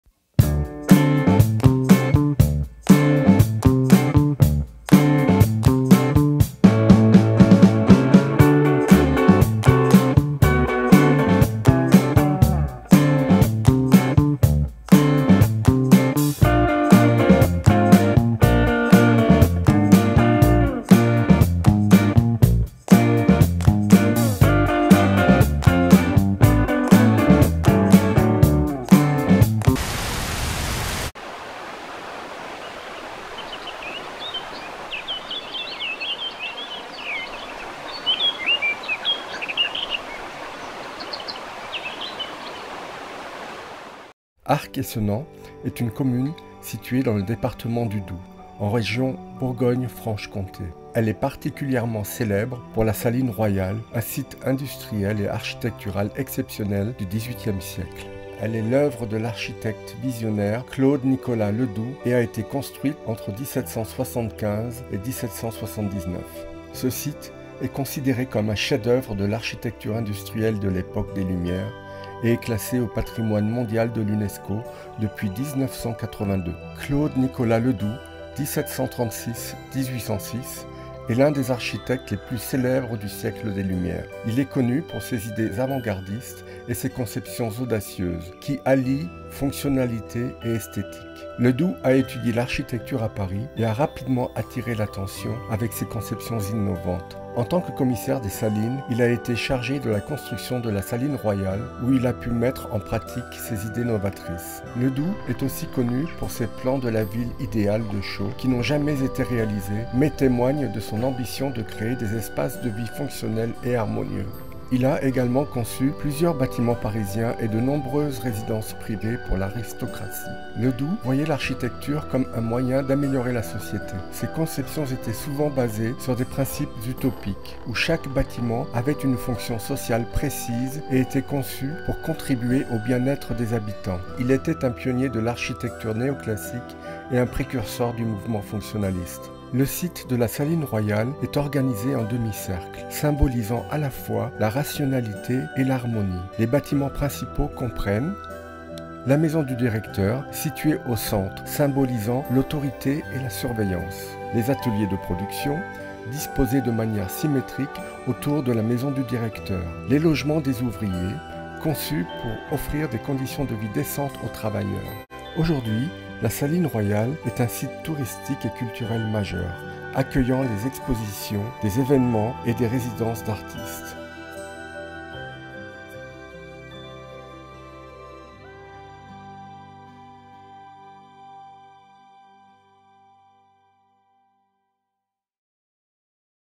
🇫🇷 🌞🌞👑 La Saline Royale d'Arc-et-Senans : Best resolution: 4K= S2160
Arc-et-Senans : Un patrimoine industriel et architectural
Arc-et-Senans est une commune située dans le département du Doubs, en région Bourgogne-Franche-Comté, en France. Elle est particulièrement célèbre pour la Saline royale, un site industriel et architectural exceptionnel du XVIIIe siècle.

La Saline royale d'Arc-et-Senans
La Saline royale d'Arc-et-Senans est une ancienne manufacture de sel créée à la fin du XVIIIe siècle. Elle est l'œuvre de l'architecte visionnaire Claude-Nicolas Ledoux et a été construite entre 1775 et 1779. Ce site est considéré comme un chef-d'œuvre de l'architecture industrielle de l'époque des Lumières et est classé au patrimoine mondial de l'UNESCO depuis 1982.

Contexte historique
À la fin du XVIIIe siècle, le sel était une denrée précieuse et sa production était étroitement contrôlée par le gouvernement français. Pour améliorer la production de sel, le roi Louis XV commanda la construction de la Saline royale à Arc-et-Senans, à proximité des vastes forêts de la région nécessaires pour alimenter en bois les fours de l'évaporation.

Claude-Nicolas Ledoux : L'Architecte des Lumières
Claude-Nicolas Ledoux (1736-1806) est l'un des architectes les plus célèbres du siècle des Lumières. Il est connu pour ses idées avant-gardistes et ses conceptions audacieuses, qui allient fonctionnalité et esthétique.

Carrière et œuvres majeures
Formation et début de carrière : Ledoux a étudié l'architecture à Paris et a rapidement attiré l'attention avec ses conceptions innovantes.
Commissaire des Salines : En tant que commissaire des Salines, il a été chargé de la construction de la Saline royale, où il a pu mettre en pratique ses idées novatrices.
Architecture visionnaire : Ledoux est aussi connu pour ses plans de la ville idéale de Chaux, qui n'ont jamais été réalisés mais témoignent de son ambition de créer des espaces de vie fonctionnels et harmonieux.
Autres œuvres : Il a également conçu plusieurs bâtiments parisiens et de nombreuses résidences privées pour l'aristocratie.
La vision de Ledoux
Ledoux voyait l'architecture comme un moyen d'améliorer la société. Ses conceptions étaient souvent basées sur des principes utopiques, où chaque bâtiment avait une fonction sociale précise et était conçu pour contribuer au bien-être des habitants. Il était un pionnier de l'architecture néoclassique et un précurseur du mouvement fonctionnaliste.

La Saline royale : Un projet monumental
Le site de la Saline royale est organisé en demi-cercle, symbolisant à la fois la rationalité et l'harmonie. Les bâtiments principaux comprennent :

La Maison du Directeur : Située au centre, symbolisant l'autorité et la surveillance.
Les ateliers de production : Disposés de manière symétrique autour de la maison du directeur.
Les logements des ouvriers : Conçus pour offrir des conditions de vie décentes aux travailleurs.
La conception de la Saline royale intègre des éléments symboliques et utilitaires, reflétant la vision de Ledoux d'une société organisée et harmonieuse.

Héritage et influence
La Saline royale d'Arc-et-Senans et l'œuvre de Claude-Nicolas Ledoux ont eu une influence durable sur l'architecture et l'urbanisme. Leur approche visionnaire continue d'inspirer les architectes et les urbanistes contemporains.

Aujourd'hui, la Saline royale est un site touristique et culturel majeur, accueillant des expositions, des événements et des résidences d'artistes. Elle reste un témoignage fascinant de l'architecture industrielle du XVIIIe siècle et de la vision utopique d'un des plus grands architectes de son temps.